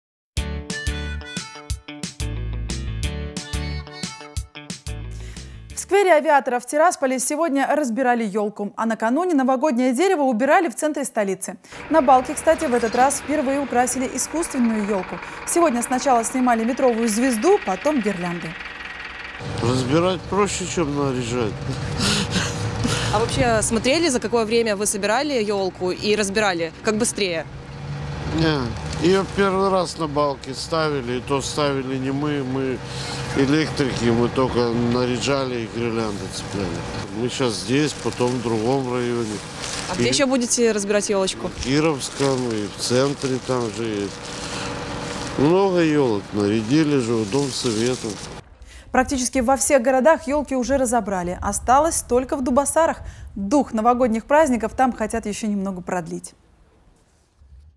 В сквере Авиаторов разобрали ёлку
В сквере Авиаторов в Тирасполе сегодня разбирали ёлку. А накануне новогоднее дерево убирали в центре столицы. На Балке, кстати, в этот раз впервые украсили искусственную ёлку. Сегодня сначала снимали метровую звезду, потом – гирлянды.